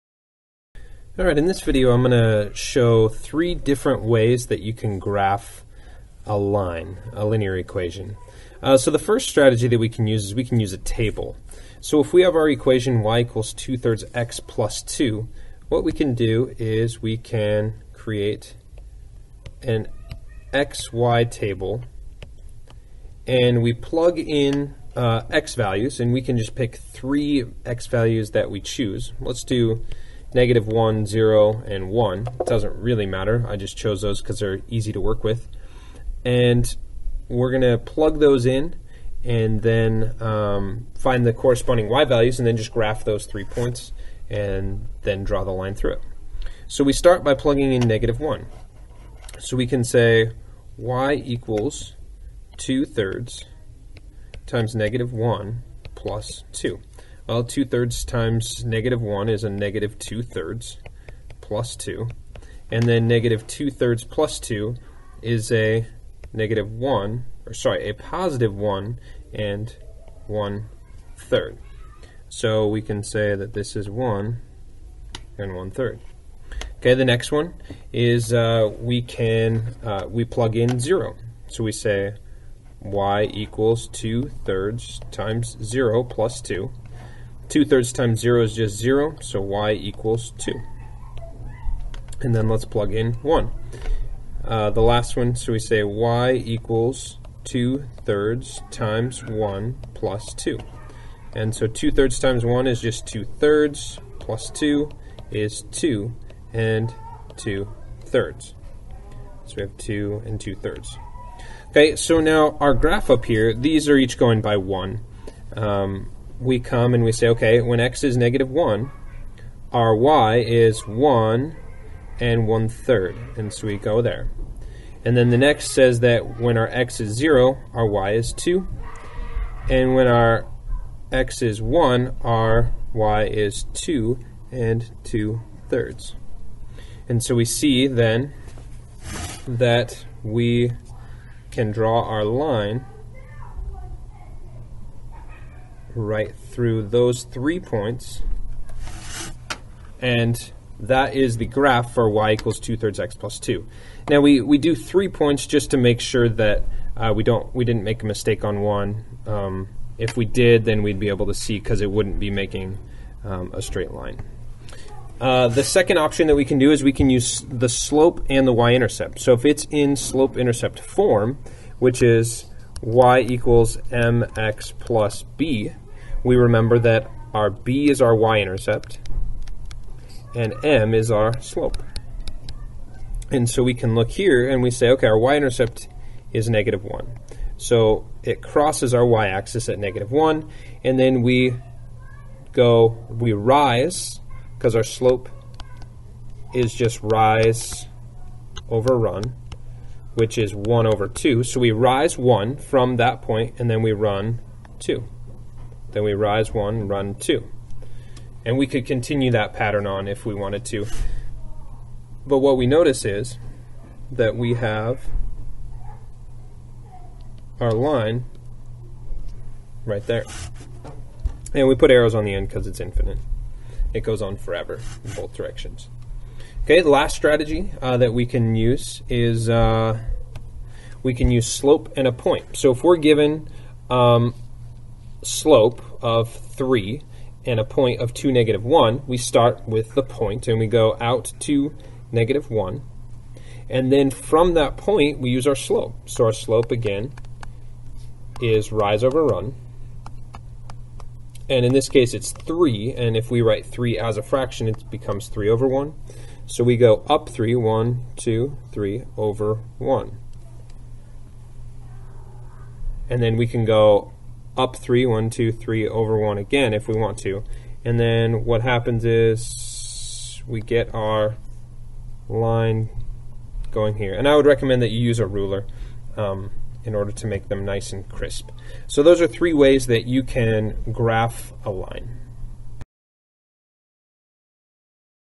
Three Strategies on How to Graph a Linear Equation
In this video I explain how to graph linear equations using three points, slope-intercept form, and slope and a point.